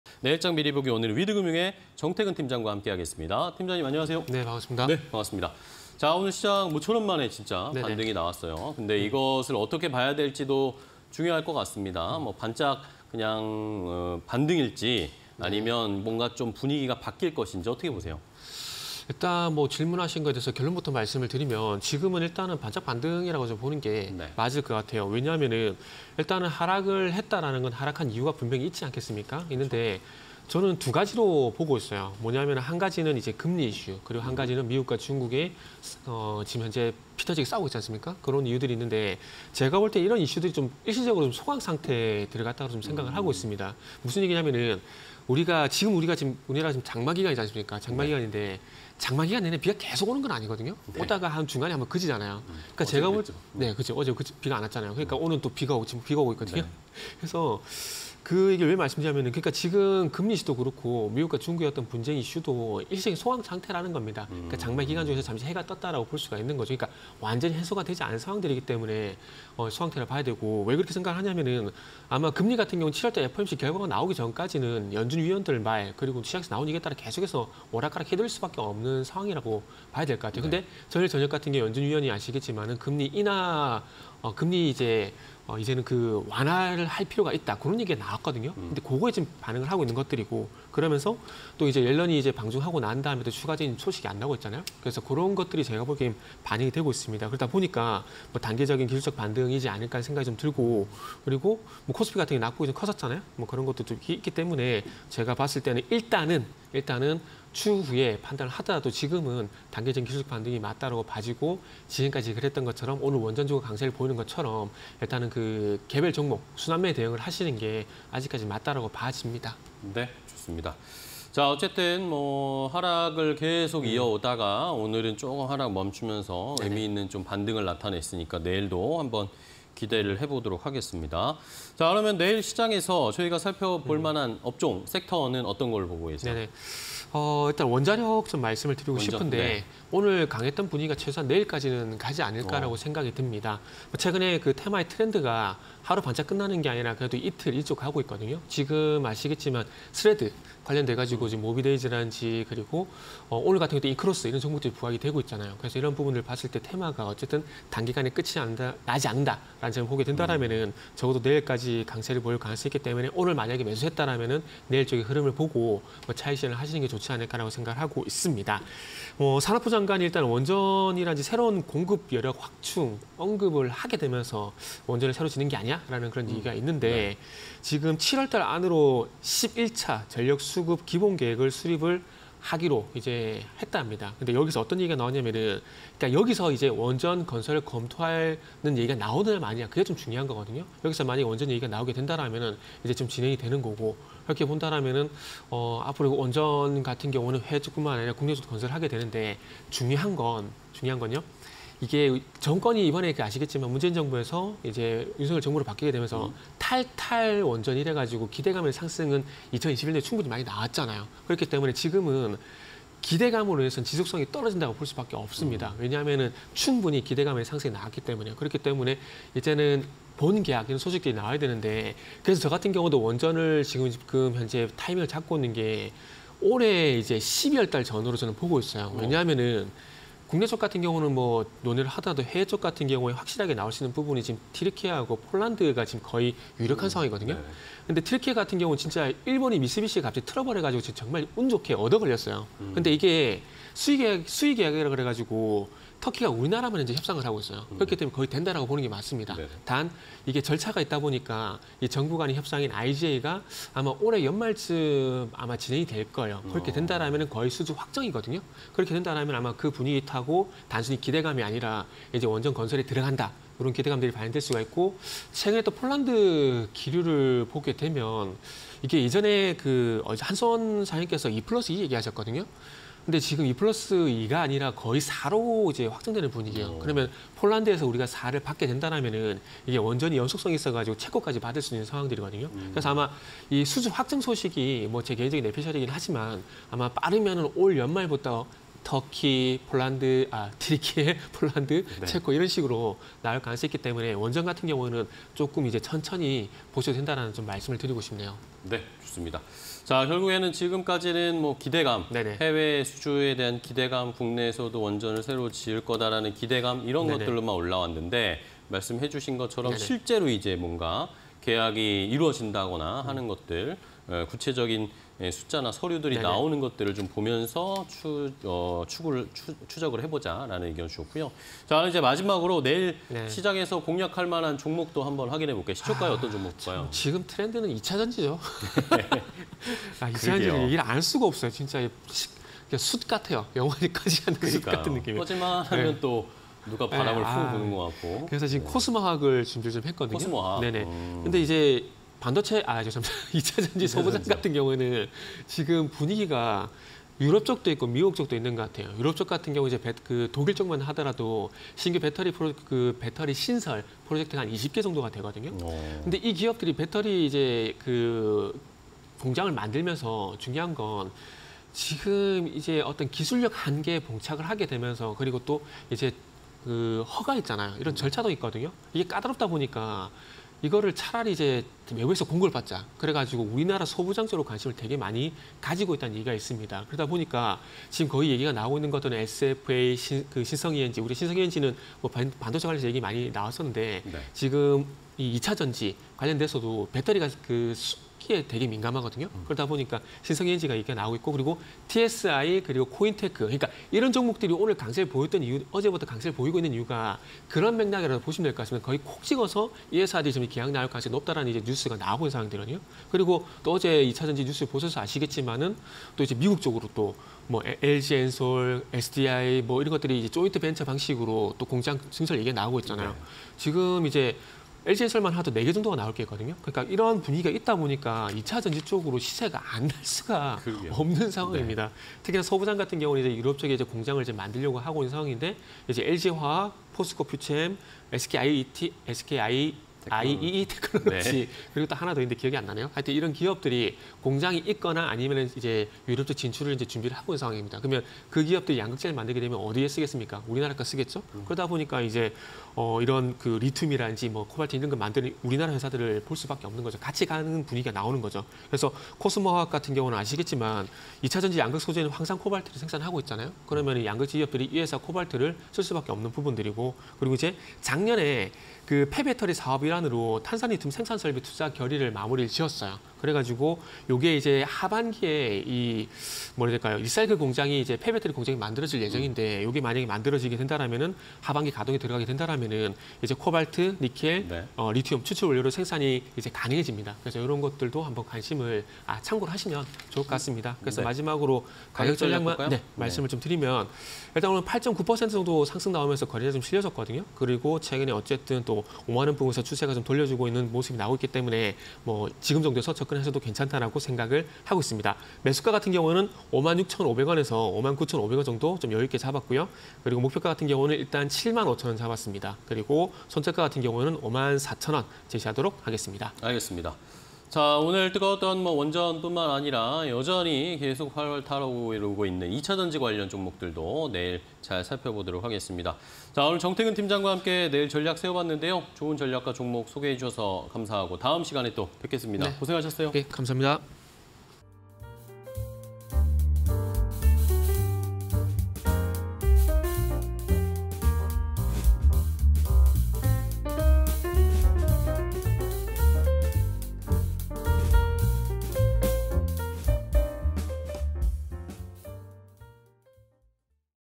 코스모화학… 배터리 밸류체인 구축 위해 신규 '소부장' 확보 집중 / 원자력… 산업부 장관, 원전 등 새 전력공급원 확충 검토 언급_내일장 미리보기 (20230711)
- 산업부 장관, 원전 등 새 전력공급원 확충 검토 언급
- 7월 안에 '11차 전력수급기본계획' 수립 착수 계획
- 새로운 SMR 기술 개발… 총 3,992억 원 투자 예정
- 관련주: 한전산업, 우진, 한신기계 등

코스모화학(005420)
- 유럽 신생 배터리 기업 확산에도 기술적 한계 봉착
- 배터리 밸류체인 구축 위해 신규 '소부장' 확보 집중
- 폐배터리 사업 일환… 탄산리튬 생산설비 투자 결의